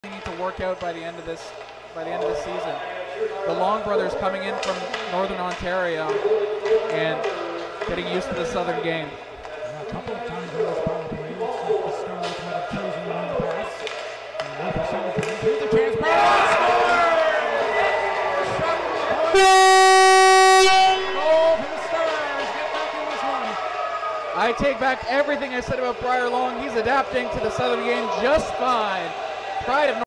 St. Thomas Stars - Briir Long 6 (Harrison Franssen, Owen van de Ven) PG vs Strathroy Rockets
Underage rookie Briir Long's sixth of the season brings the Stars back to within a goal of the Rockets.